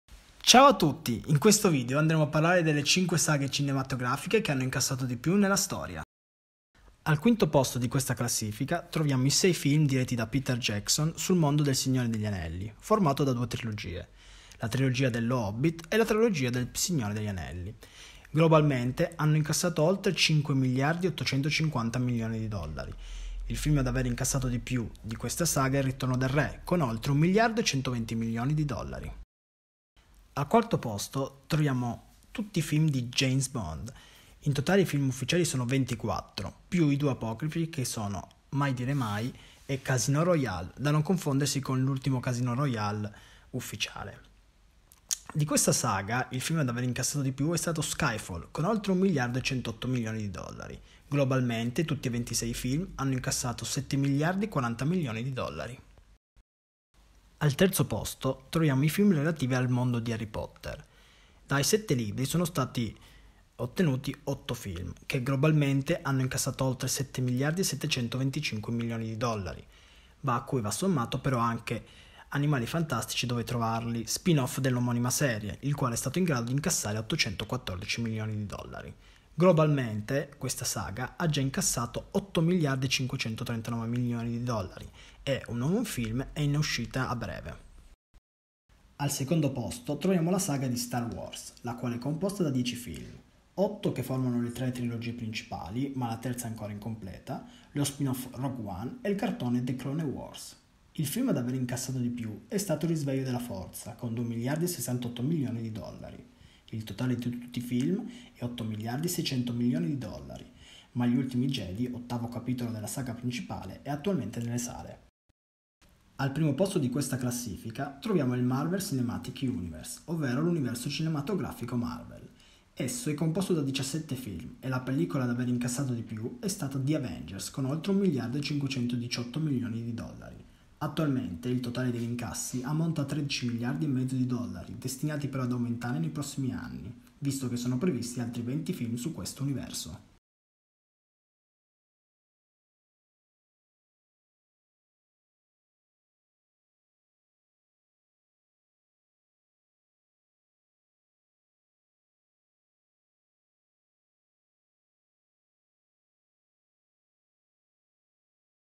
Le 5 saghe ad avere incassato di più al cinema - Le curiosità di BackToTheNerd 1
In questo video troverete la classifica delle cinque saghe ad aver incassato di più nella storia del cinema.